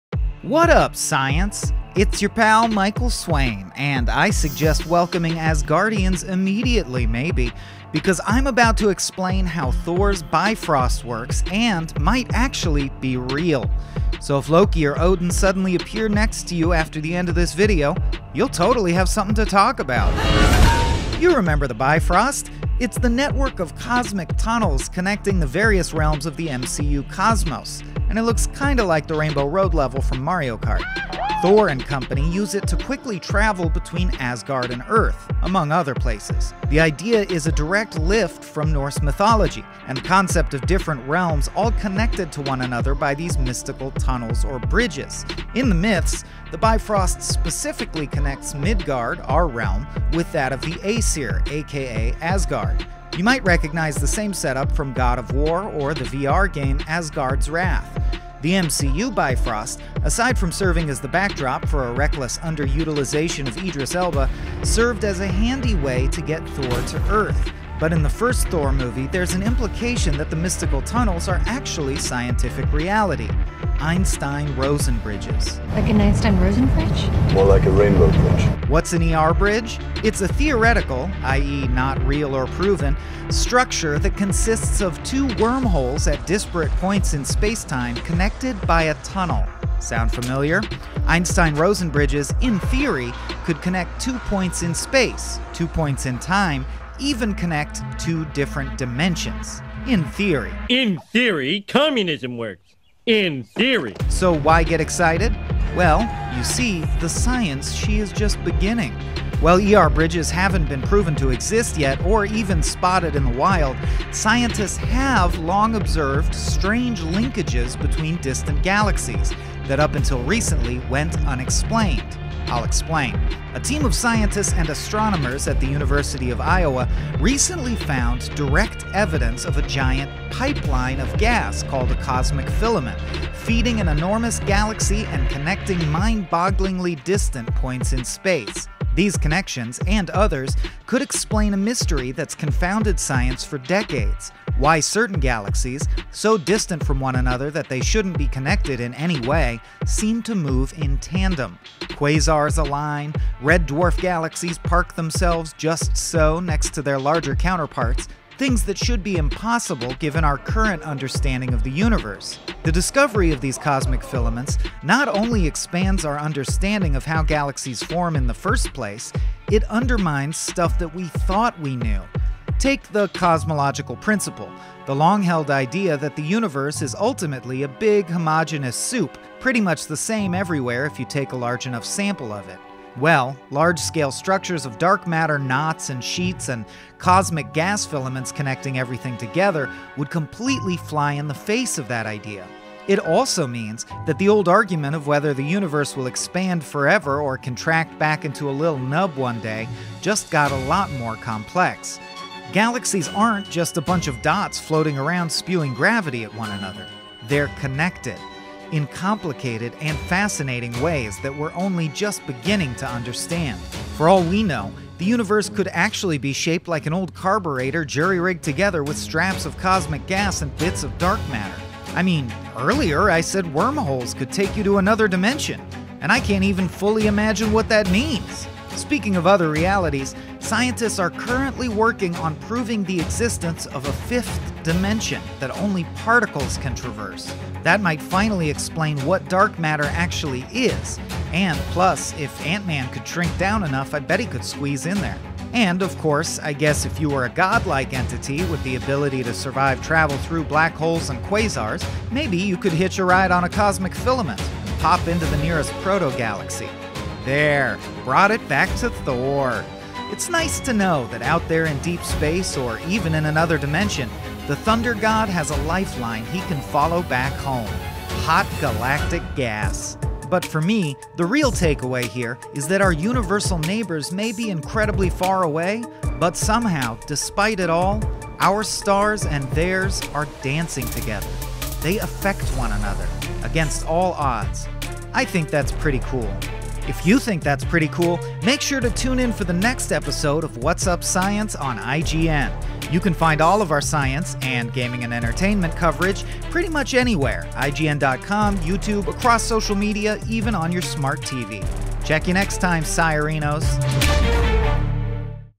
The Bifrost From Thor Might Actually Be Real - What Up Science?
Have you ever wondered if the Bifrost, that magic rainbow bridge that links Earth 616 and Asgard in the MCU and various depictions of Norse mythology...WAS REAL? NO?! Too bad! Science doesn't care, and it turns out vast superstructures of dark matter are creating spooky linkages between distant galaxies and even feeding the birth of new ones. Can we drive down them? Not yet, but science is a tricky devil and you never know where we'll go from here. So let's learn a bit about how galaxies dance, how it flies in the face of everything we thought we knew about the shape of the universe, and how we're all a little more connected that you might think. Step aside, Heimdall, it's time to open the Bifrost.